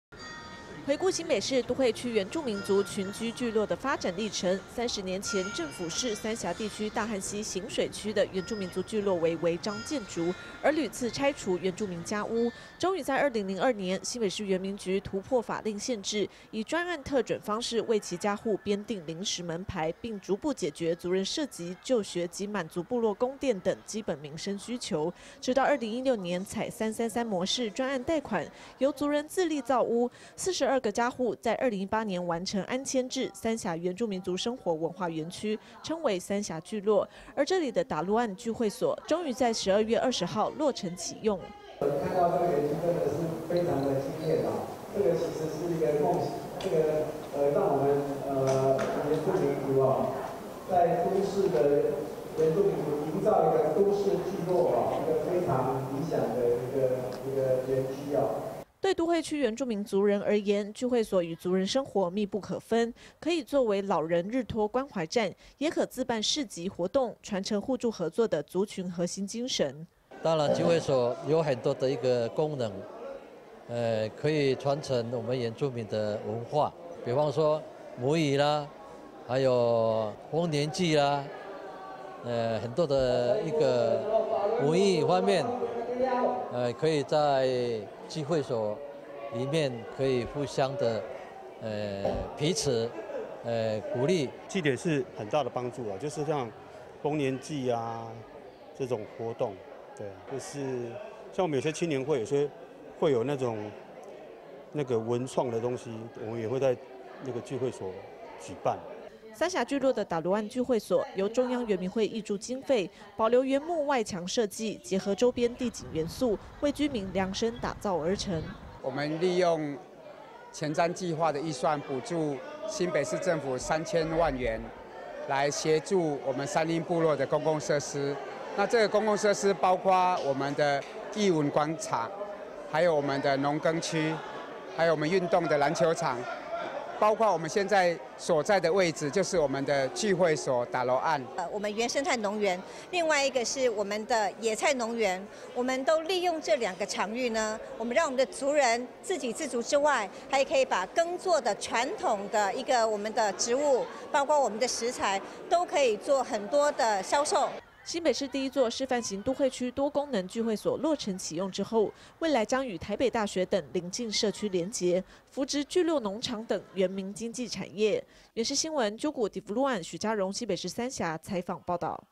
首座示範型都會聚會所 三峽talo'an正式啟用 2020-12-21 IPCF-TITV 原文會原視新聞
回顧新北市都會區原住民族群居聚落的發展歷程，30年前政府視三峽地區大漢溪行水區的原住民族聚落為違章建築，而屢次拆除原住民家屋。終於在2002年，新北市原民局突破法令限制，以"專案特准"方式為其家戶編訂臨時門牌，並逐步解決族人設籍、就學及滿足部落供電等基本民生需求。直到2016年採"三三三模式"專案貸款，由族人自立造屋，42家戶在2018年完成安遷至"三峽原住民族生活文化園區"，稱為"三峽聚落"，而這裡的talo'an聚會所，終於在12月20號落成啟用。

對都會區原住民族人而言，聚會所與族人生活密不可分，可以作為老人日托關懷站，也可自辦市集活動，傳承互助合作的族群核心精神。

三峽聚落的talo'an聚會所，由中央原民會挹注經費，保留原木外牆設計，結合周邊地景元素，為居民量身打造而成。

新北市第一座示範型都會區多功能聚會所落成啟用後，未來將與台北大學等鄰近社區連結，扶植聚落農場等原民經濟產業。